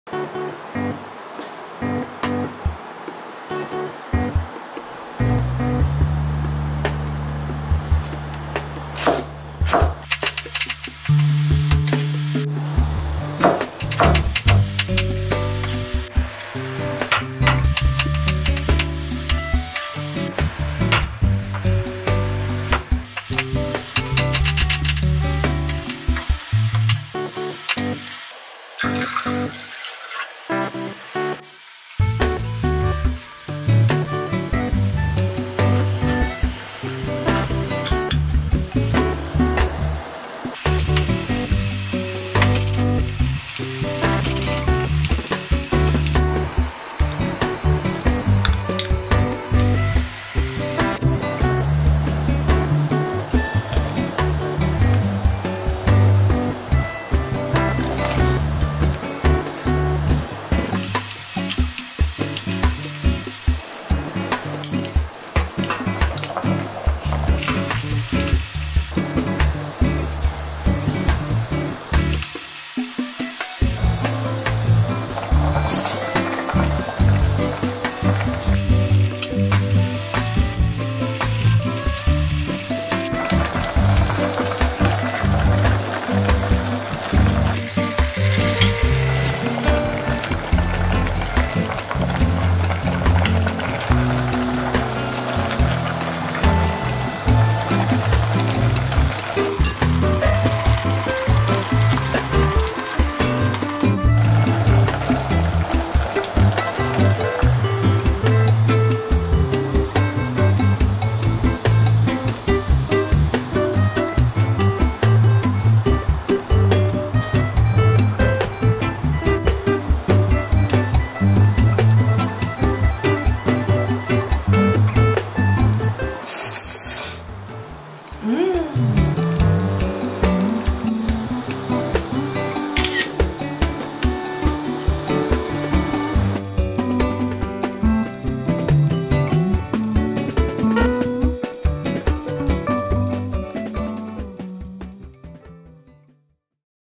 [Eng] 고깃집 된장국수 만들기 Soybean Paste Noodle Soup (doenjang guksu)
맛있는 고깃집에는 빠지지 않는 메뉴! 된장국수를 집에서 만들어보세요
된장찌개만큼 쉽고 맛있어요💞
영상 맛있게 보시고, 궁금한 게 있으면 댓글 남겨주세요 :)

Try making soybean paste noodle soup at home that you can have at the Korean BBQ restaurant.
It's as easy & delicious as a soybean paste stew💞

[Ingredients / 재료]
kimchi / 김치 50g
beef / 소고기 80g
siitake mushrooms / 표고버섯 2 EA
zucchini / 애호박 ½ EA
green onion / 대파 ½ EA
noodles / 국수면 140g
dashi / 육수 700ml

[for seasoning sauce / 양념]
soybean paste / 된장 2T
chili powder / 고춧가루 1T